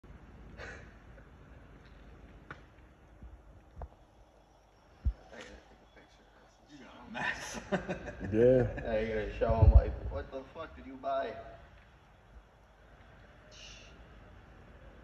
Bad hub wheel seal and brake/drum. On a T680 kenworth
Bad wheel hub and seals on this T680 Kenworth, brakes,wheel hub, and drum.